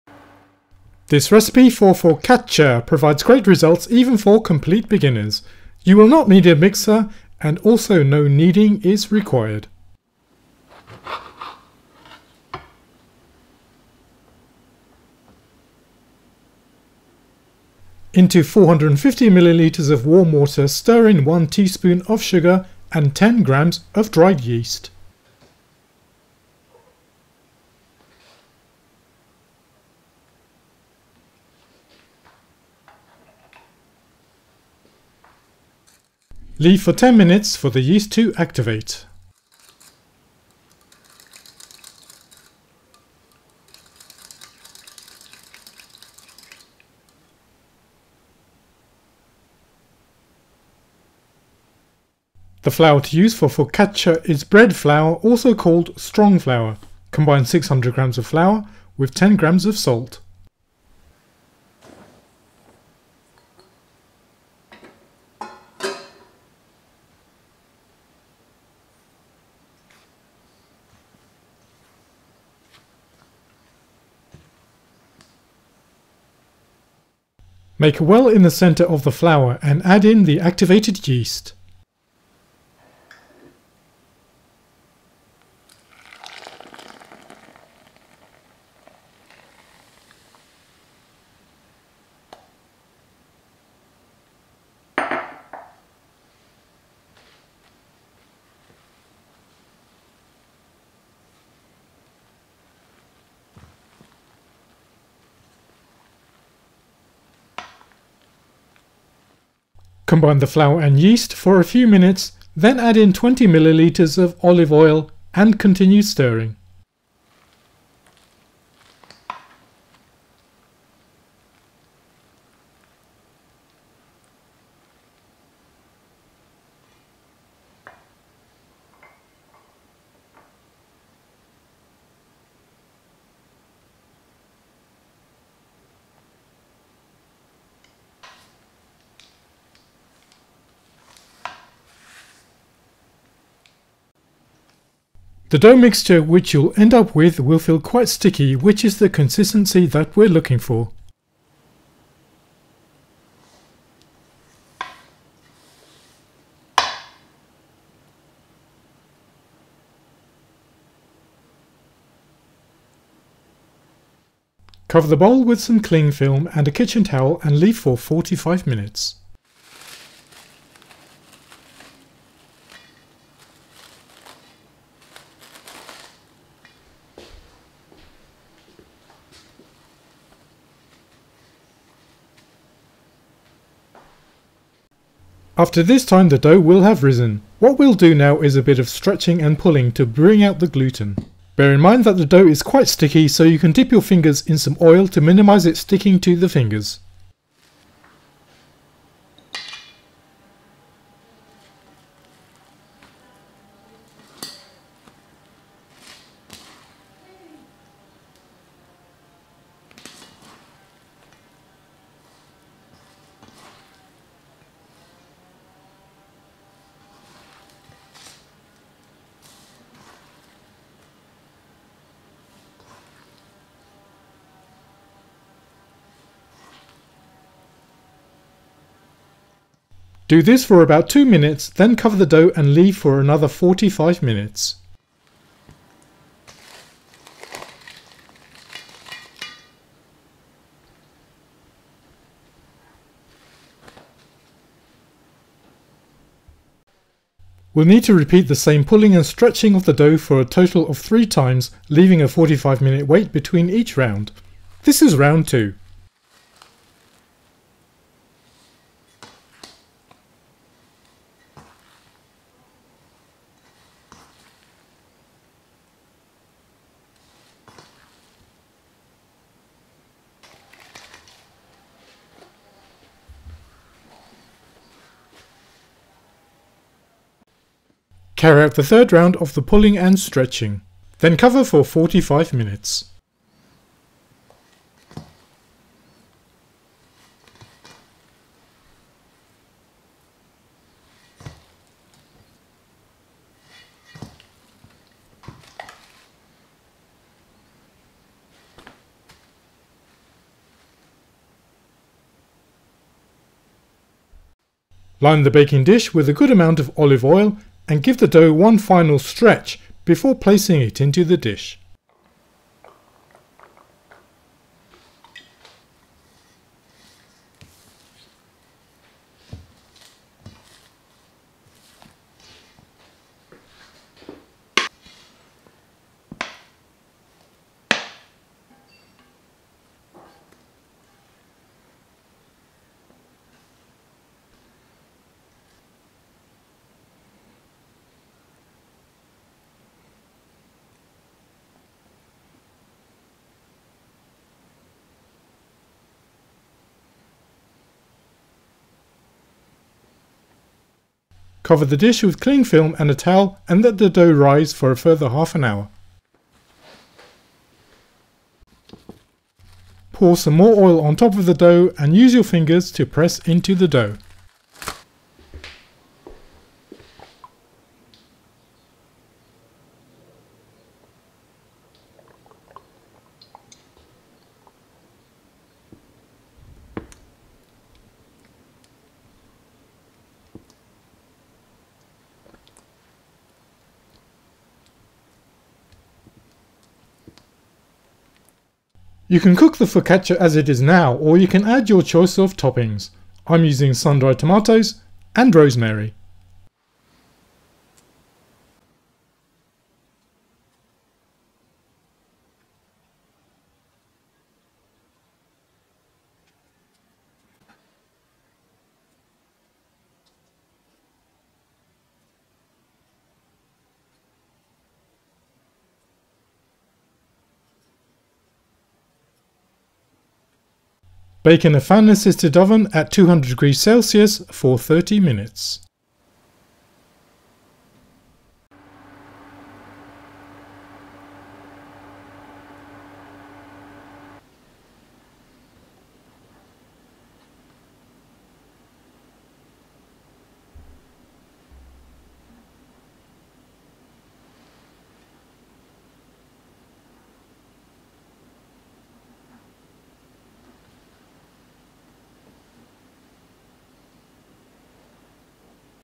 Soft and chewy Focaccia bread with sundried tomatoes and rosemary
How to make focaccia bread with rosemary and sundried tomatoes without kneading or a mixer.  It is easy to make and suitable for beginners.

Ingredients: 600g strong flour, 450ml warm water, 10g salt, 8g yeast, 20ml olive oil, rosemary, sundried tomatoes.
Bake at 200C for 30 minutes